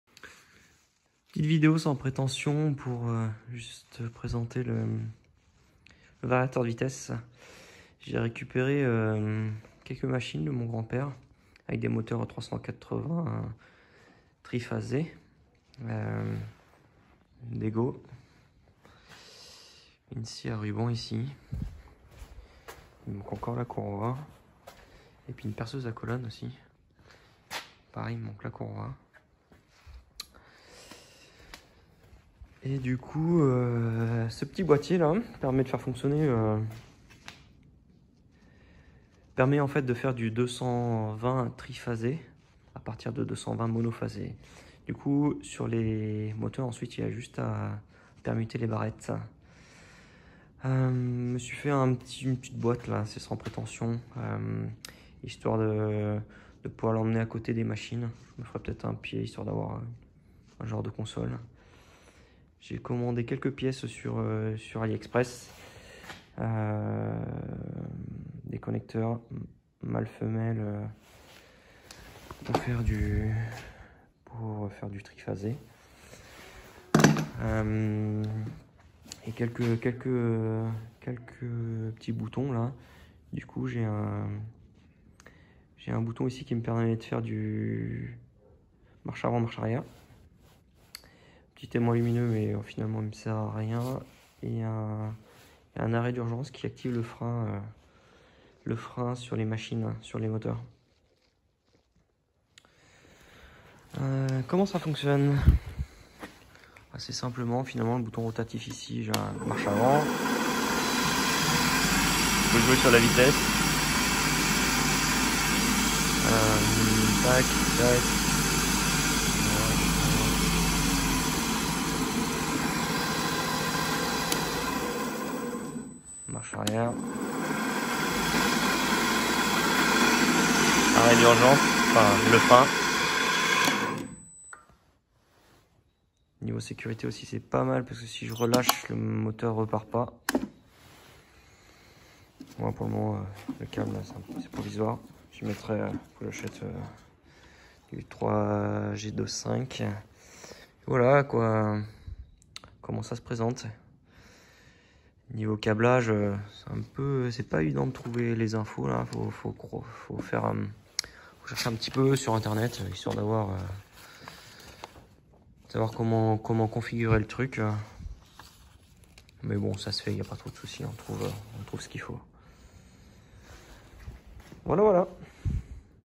Faire Fonctionner un moteur 380v tri avec du 220v mono
Petite présentation de mon installation permettant de faire fonctionner des moteurs 380v triphasé via un variateur de vitesse alimenté en 220v mono.

Variateur de vitesse : Schneider ATV12HU22M2